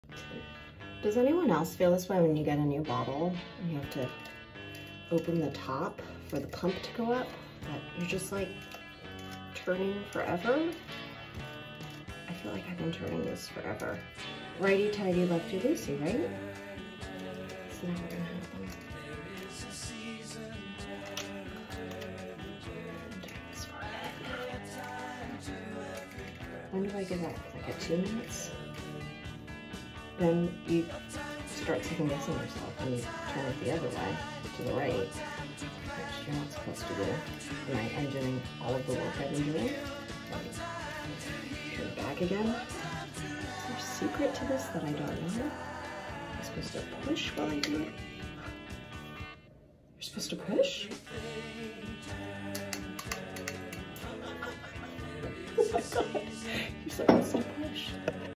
I was today-years-old when I learned to open a bottle w a pump on it
BASIC LIFE SKILL: Opening a bottle of liquid soap! I'm always learning...